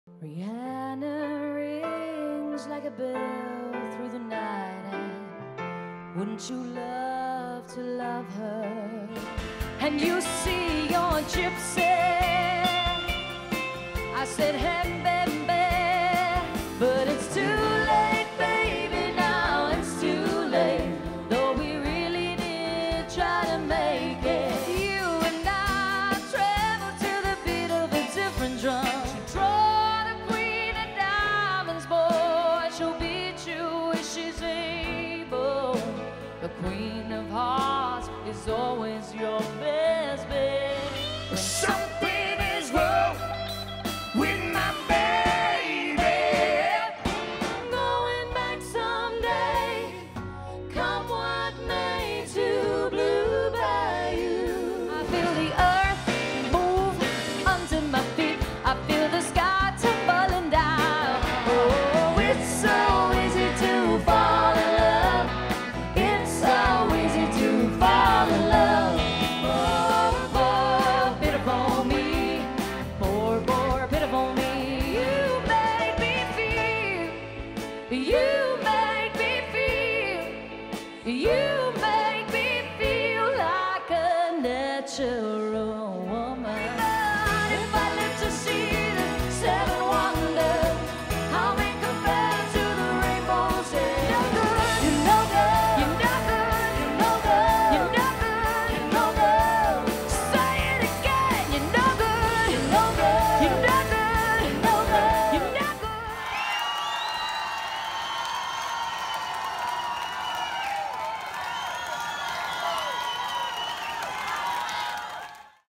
Bloom Sings - Stevie Nicks, Linda Ronstadt, Carole King Songbook - live show footage 2021
Here is a recording taken live from the desk and footage from our Stevie Linda Carole songbook show. Touring Australia and New Zealand 2021, go or Facebook - Bloom Official Music for show dates. Xx